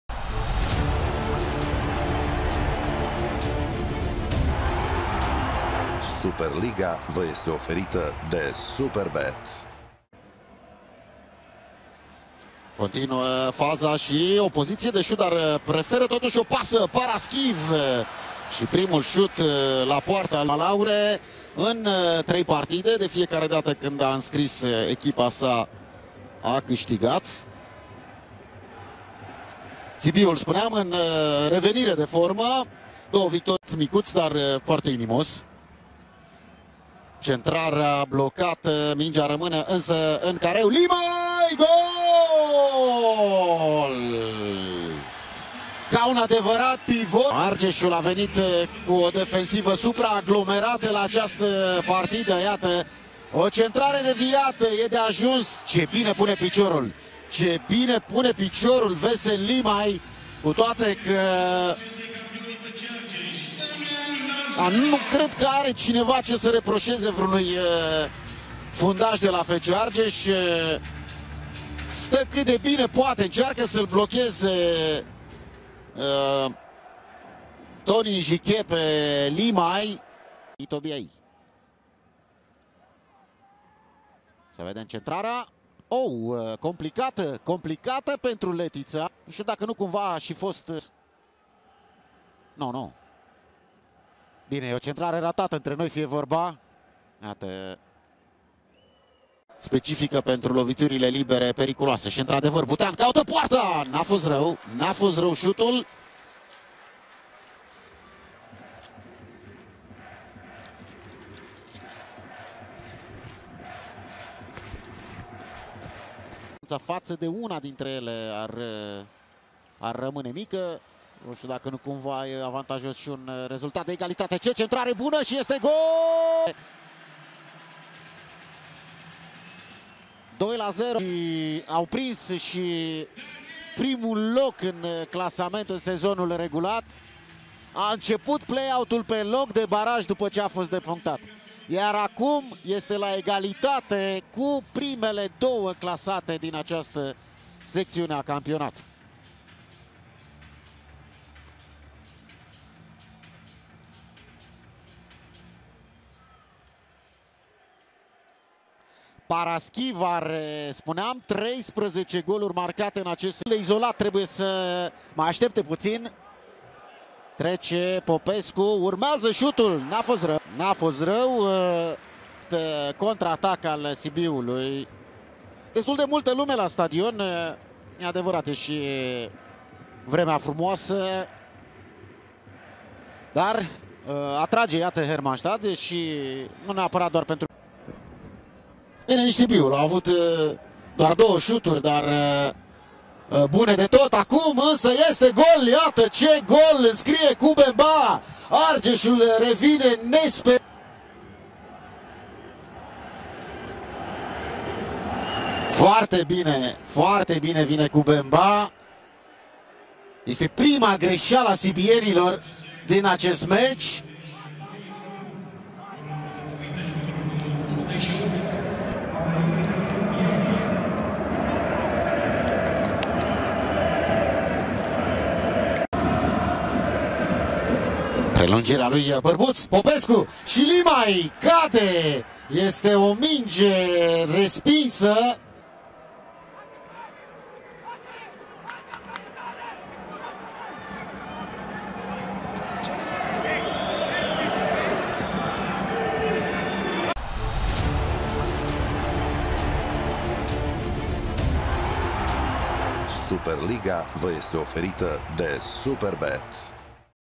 REZUMAT | Hermannstadt – FC Argeș 2-1. | Play-Out, Etapa 5, SuperLiga, 2022 – 2023
Au marcat: Limaj 11, Paraschiv 38 / Koubemba 61